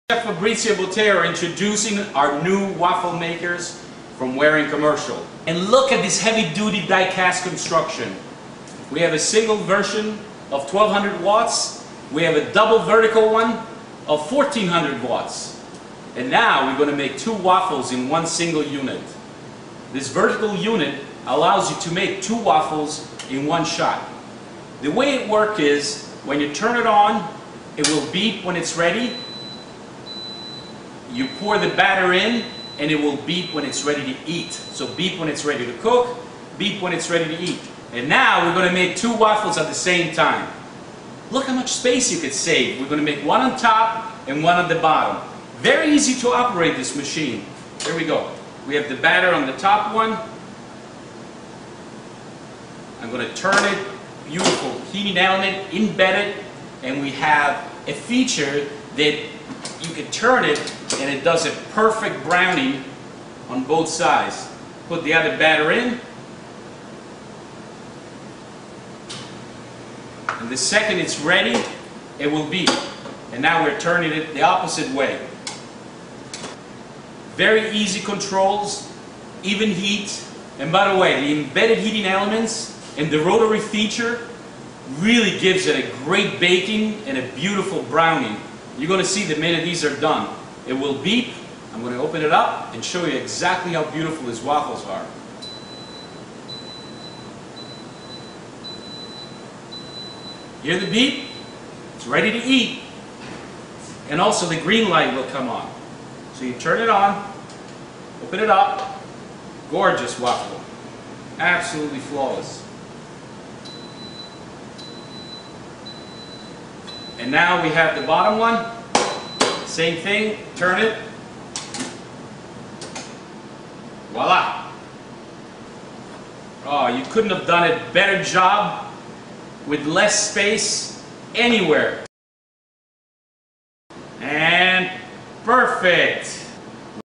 Waring WW180K and WW200K Belgian Waffle Maker
Waring Commercial has taken quality and ingenuity to new levels with the latest extension of our waffle maker line. Whether you need a single or double waffle maker, Waring provides commercial-grade housing and heating components that are unmatched in the industry. The nonstick plates have heating elements built in for maximum heat distribution, offering the exceptional performance you can expect with every Waring product.